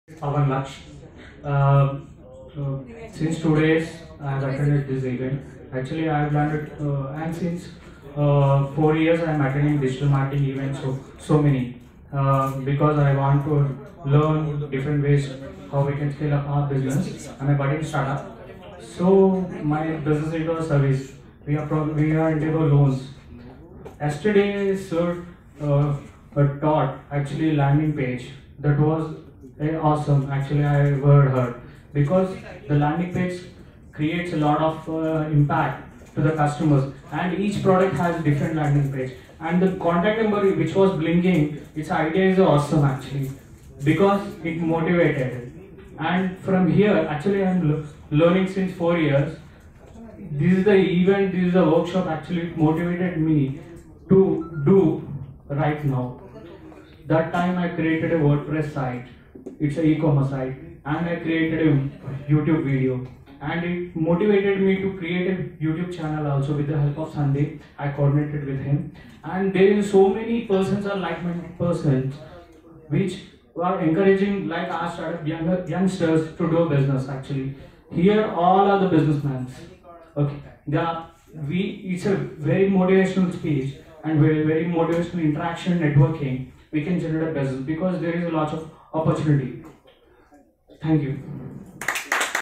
Advanced E Commerce & Digital Marketing Workshop Testimonial for MD Ilias by Pavan Kumar Naini
Advanced E-Commerce & Digital Marketing Workshop Testimonial for Mohammed Ilias by Pavan Kumar Naini